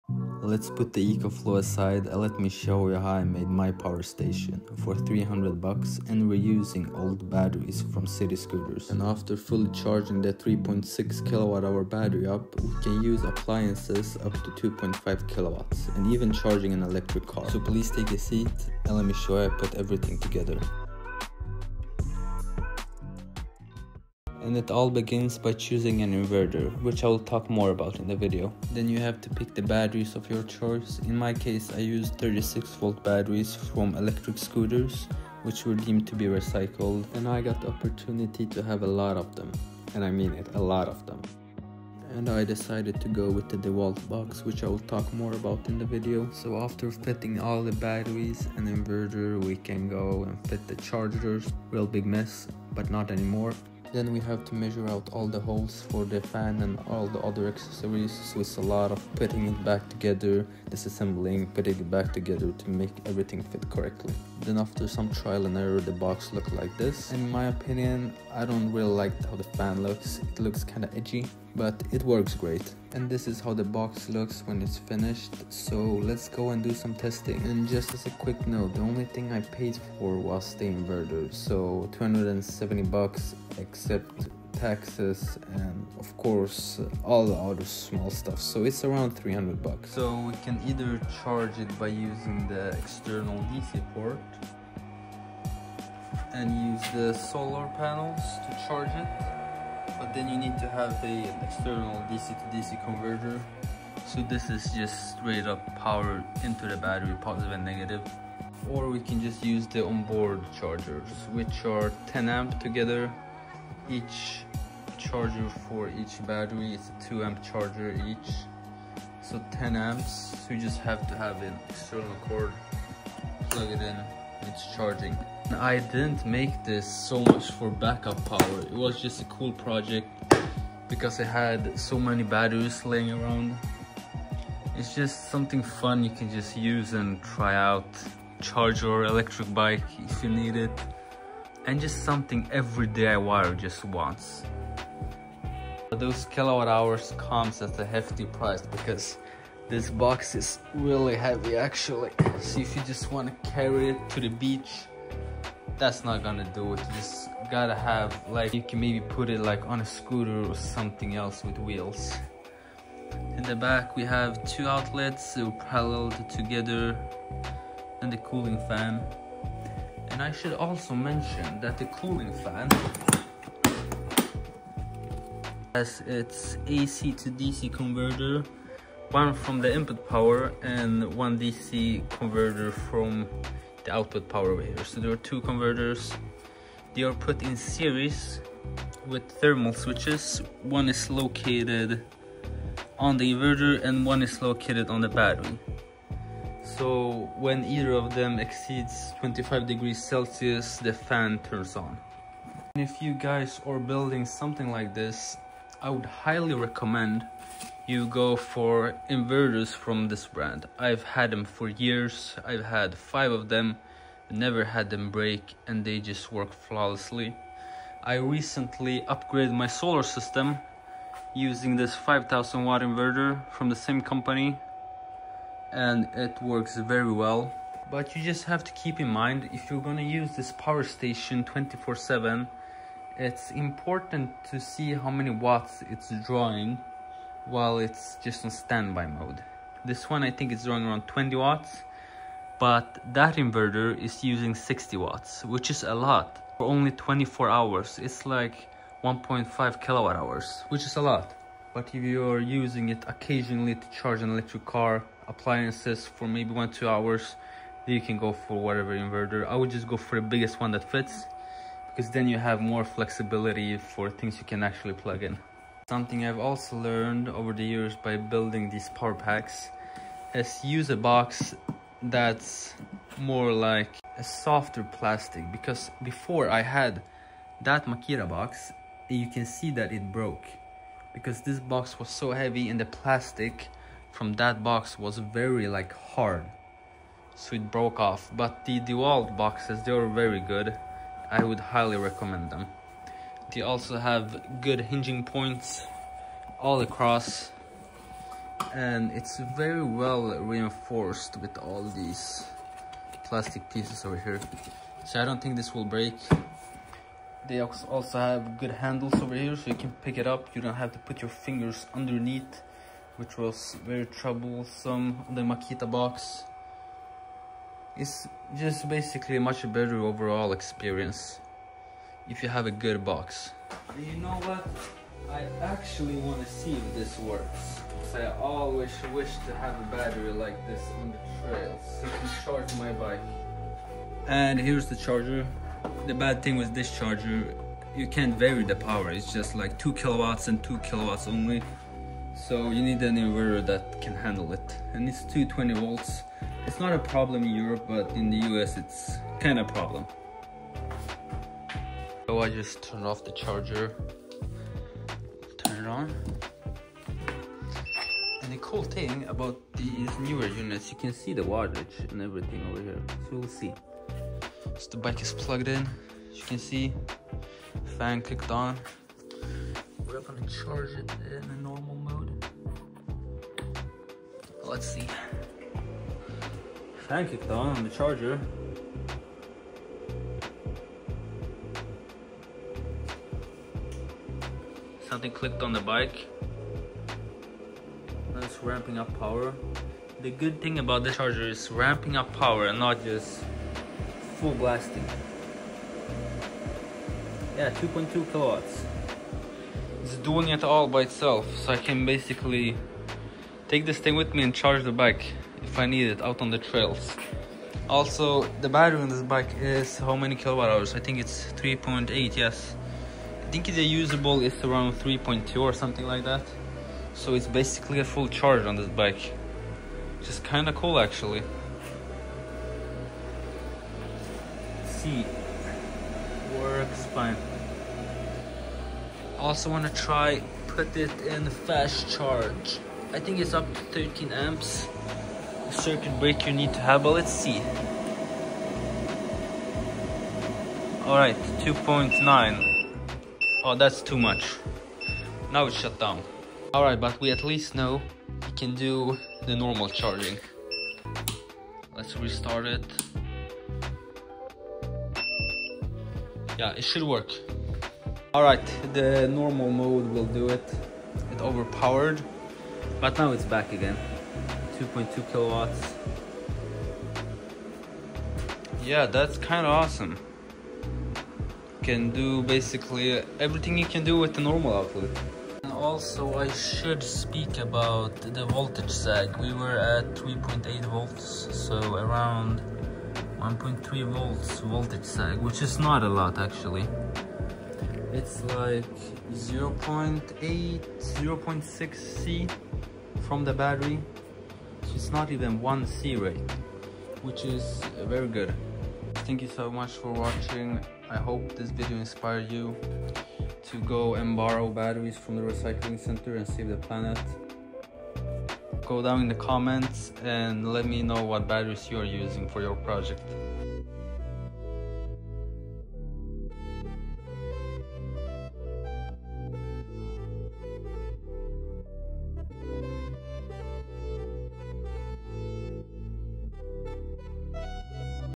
DIY $300 Battery Powered Generator - Using recycled batteries from city scooters !
DIY $300 Portable Power Station – Power everywhere !
Want a battery-powered generator that can charge your electric car, phone, laptop, and more? In this video, I show you how I built a 3,6kwh portable power station using recycled lithium-ion batteries, and a 230V inverter. This power bank can even charge an electric car off-grid!

In this video I give you some inspiration of how your power station  can look like? we go through Real-world charging test with an electric bike and how we managed to fit inside a budget of $300

What do you think of this DIY power station? Would you use this for EV charging or as a backup generator?